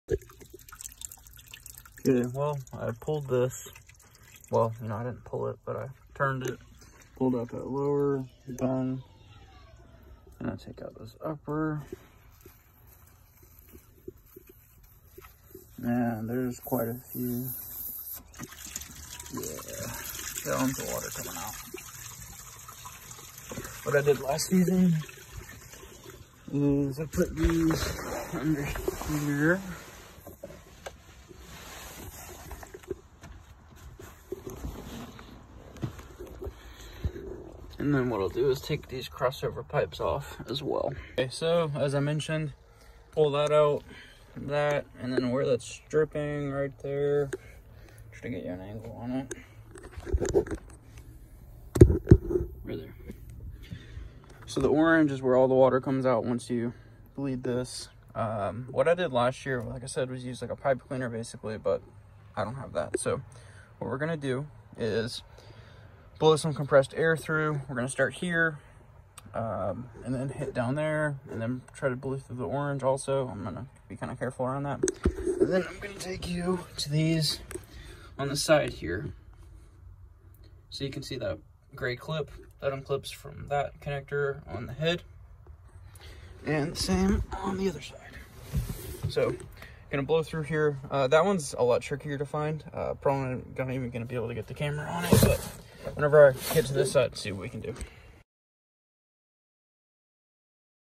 Winterizing my Mercruiser 5.0 inboard boat engine. Single point drain and draining the heads.
I like to use a pipe cleaner but was unable to locate it. Using compressed air, draining the heads, and draining the single point drain should suffice and not make a mess with Rv antifreeze.

If the water running out was not clear and looked rusty id be a bit more concerned.